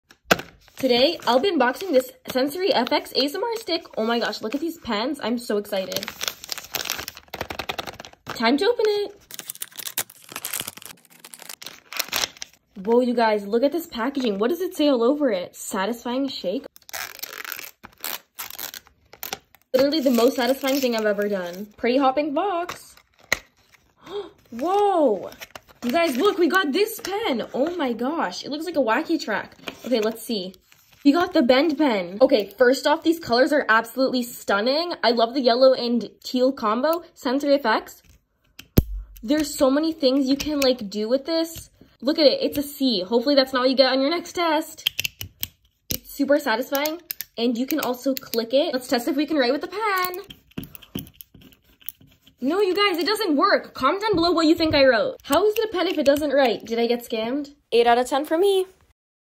*SCAMMED?* Unboxing Sensory FX ASMR Fidget Pen! *highly satisfying fidget asmr* 😍🎧
UNBOXING SENSORY FX ASMR Fidget Stick! *highly satisfying fidget asmr* 😍🎧

In this fidget shorts, I unbox a sensory fidget ASMR pen. It is super satisfying and stress relieving to play with. It looks like a wacky track fidget toy! Comment if you would buy fidgets like these :)

Vid inspo: Shanny pop